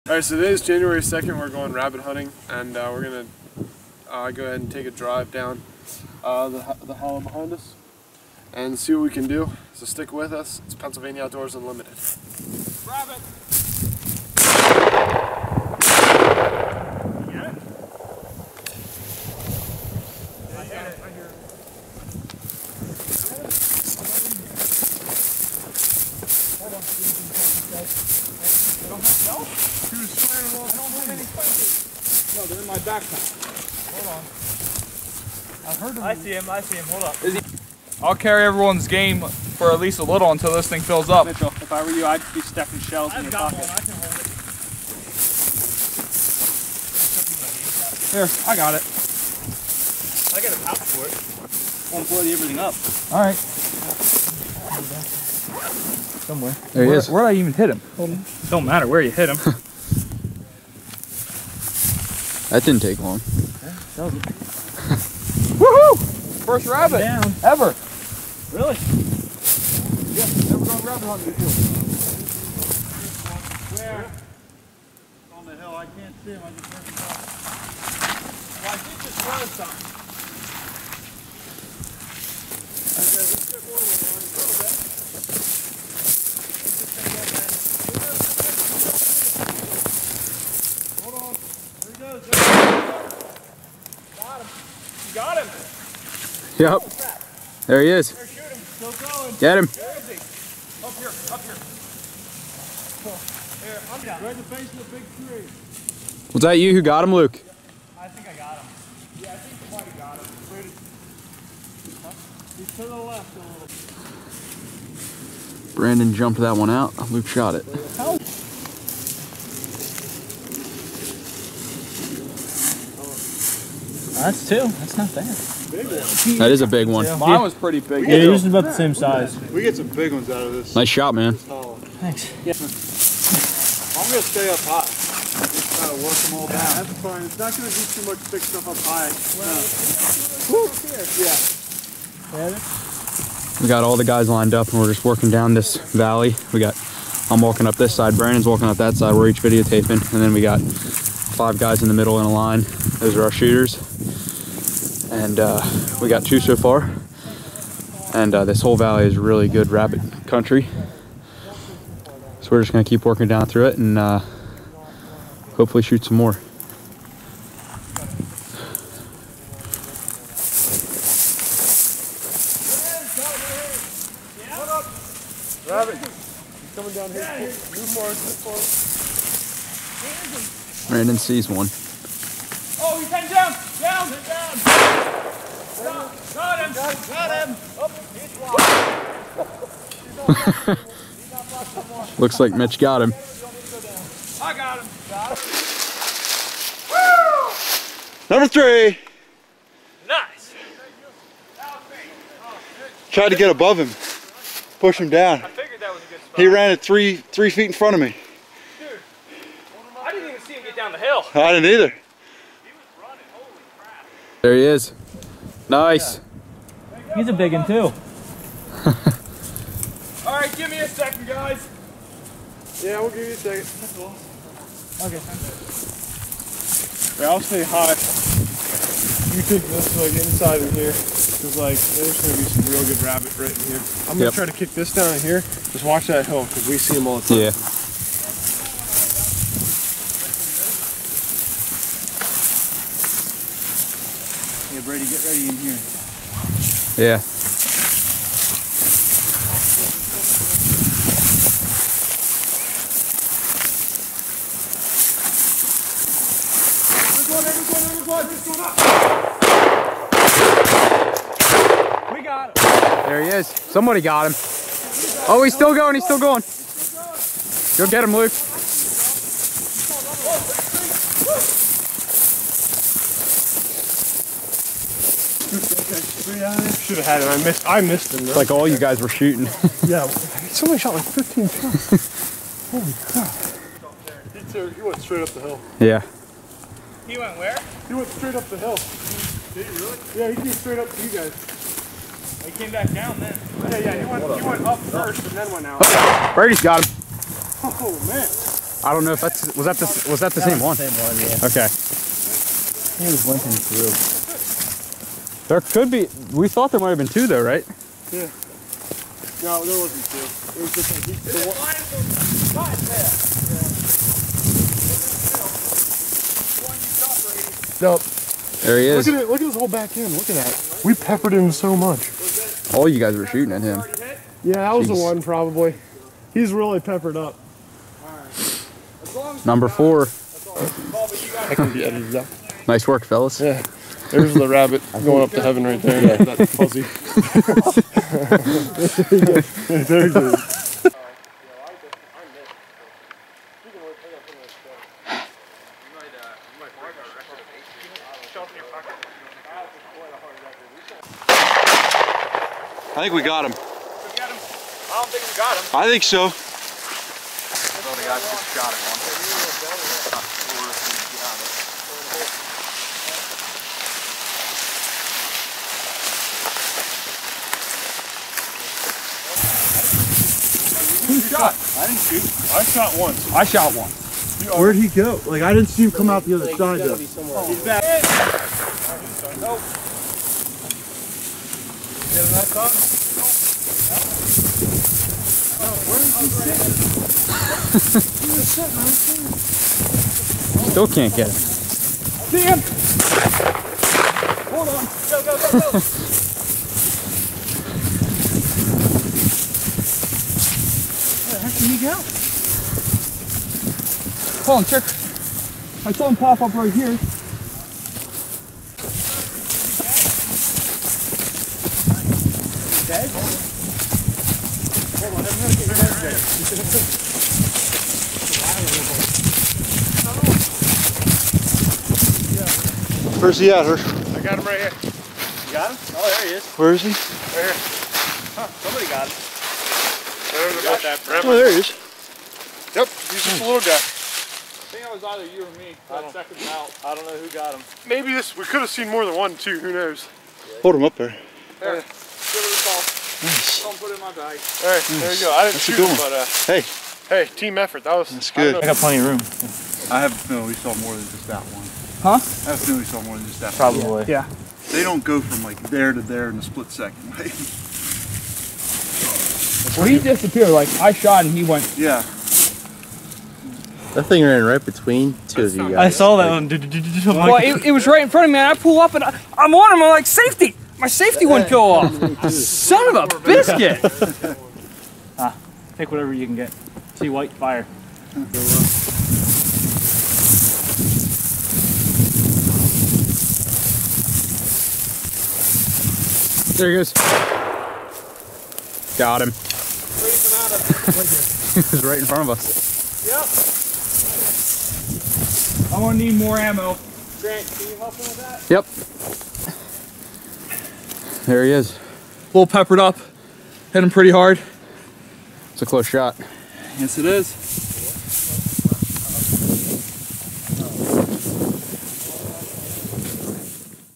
Rabbit Hunting Pennsylvania 2016
The guys hunt for some cottontail rabbits in the brush covered valleys of Bedford County, Pennsylvania.